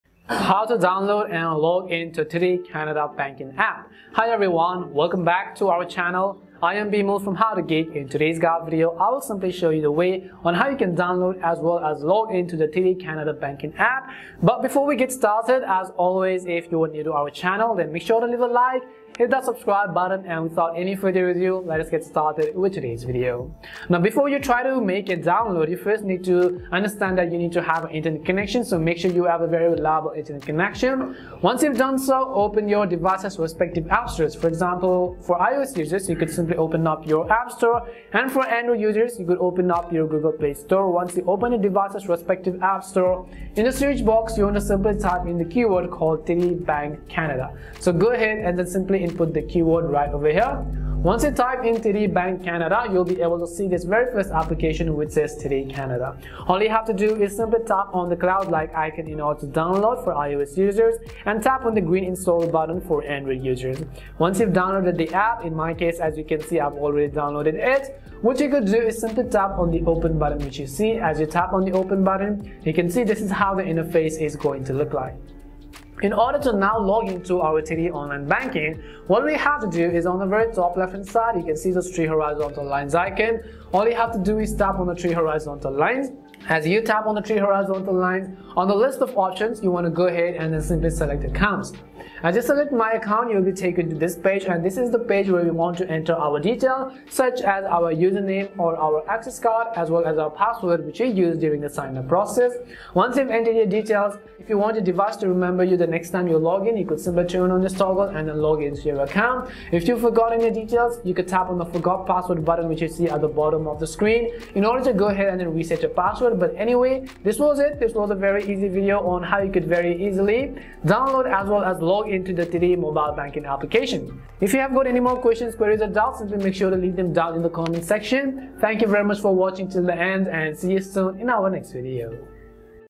How to Download & Login to TD Bank (Canada) App
In this tutorial video, I will quickly guide you on how you can download and log in to the TD Canada Banking App. So make sure to watch this video till the end.

1) Firstly, make sure you are connected to the Internet connection
2) Open up your device's App Store
3) Search for TD Bank (Canada)
4) Tap on the download icon in order to download
5) Now open the app after you download it
6) Tap on My Accounts
7) Enter your login details
8) This way, you can download and login to TB Bank (Canada) App

0:00 Introduction
0:24 How to Download
1:16 How to Login
1:56 Outro: Final Verdict